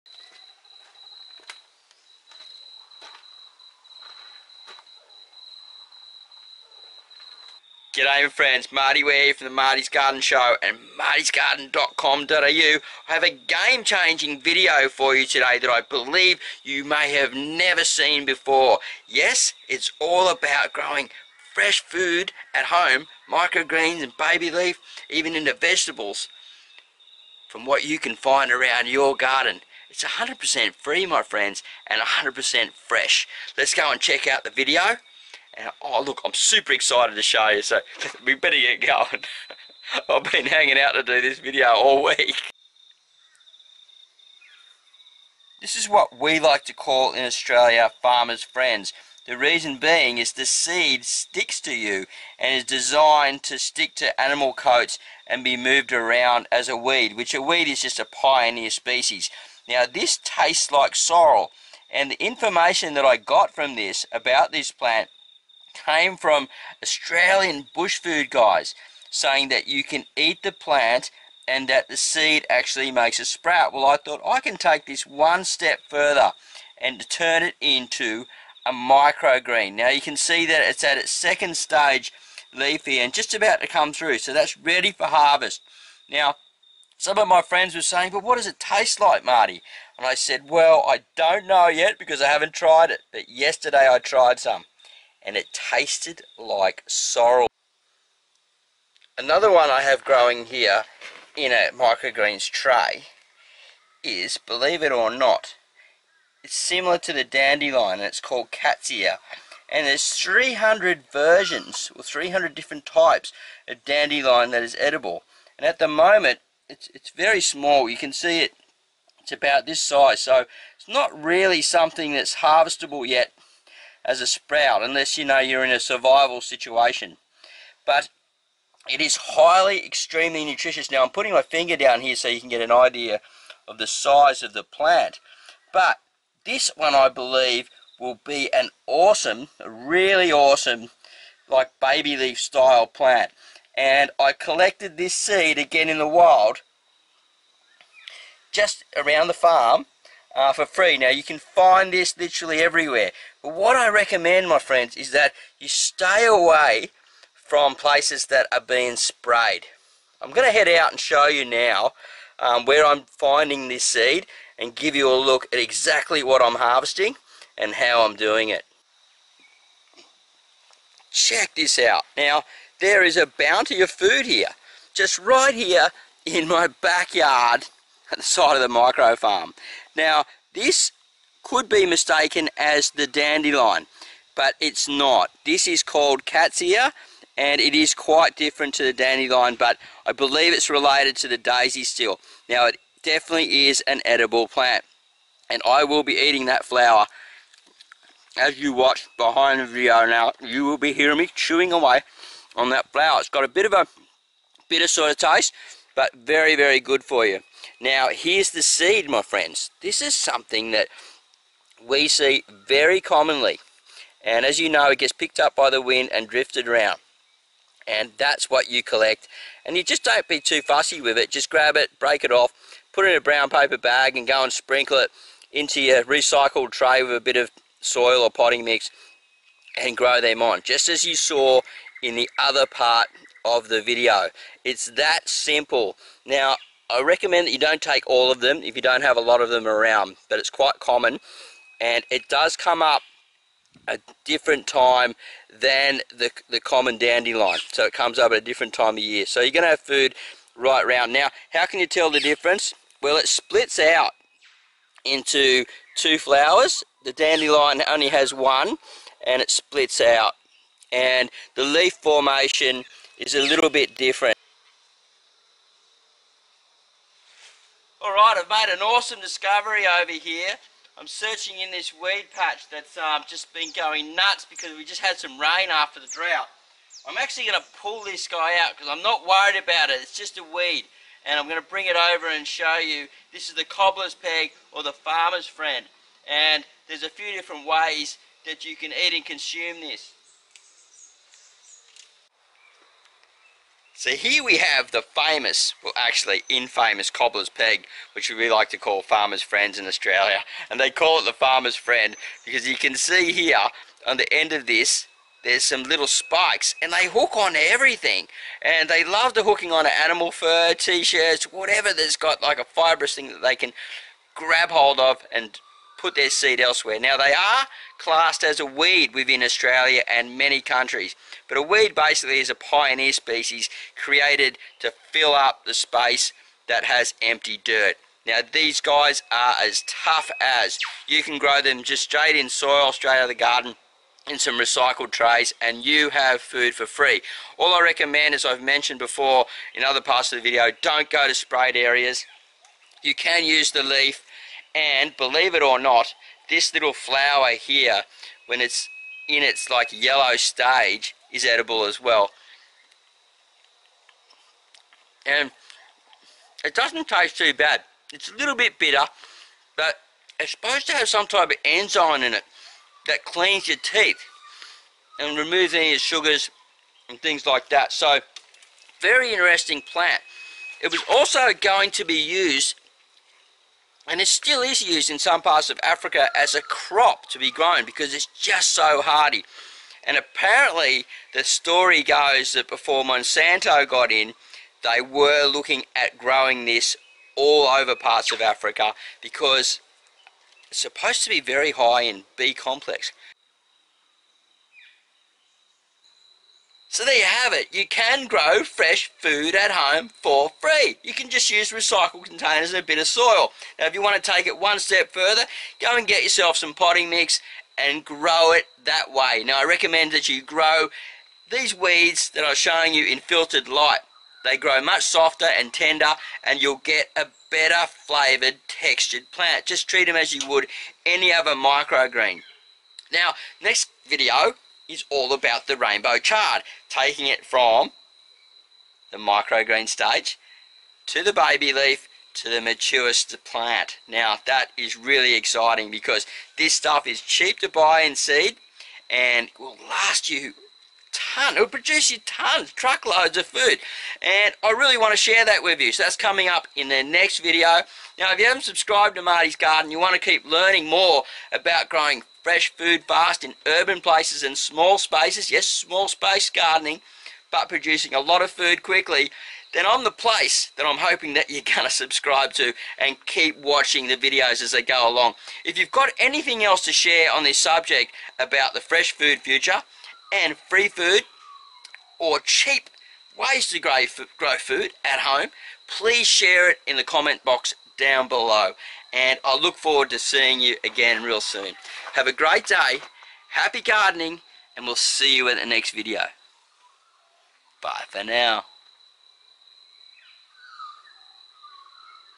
Harvest Free Food Microgreens Growing Wild
Free food, is it possible?
It sure is once you get some basic knowledge behind you.  I am always trying to push new boundaries and create new ways to grow fresh food at home fast.
My passion and drive has always been around small space gardening.